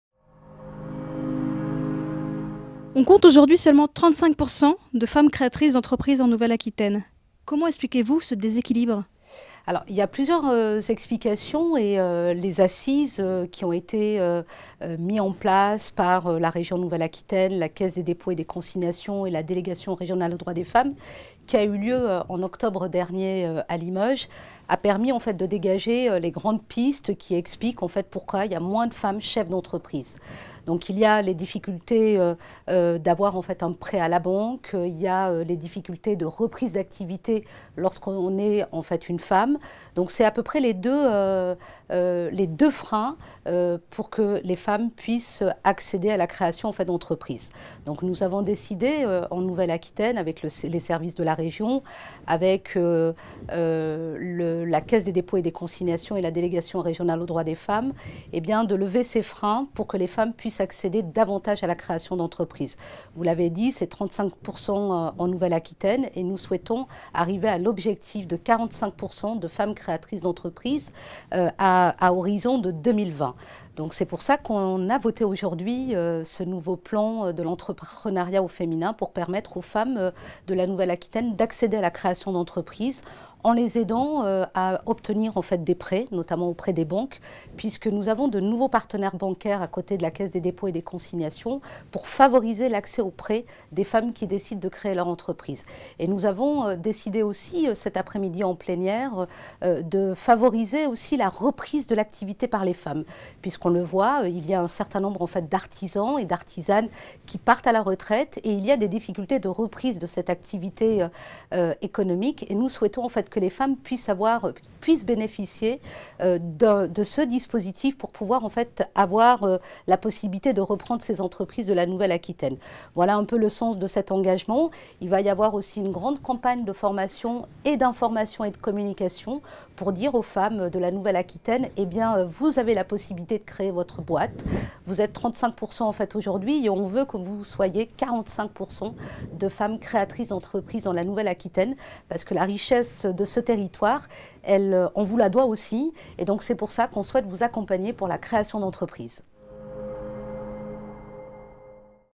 Naïma Charaï - Plan d'action régional en faveur de l'entrepreneuriat des femmes
Naïma Charaï, Conseillère régionale, déléguée à l'égalité femmes-hommes, à la lutte contre les discriminations et à la solidarité au Conseil régional de Nouvelle-Aquitaine, revient sur le Plan d'action régional en faveur de l'entrepreneuriat des femmes en nouvelle-Aquitaine.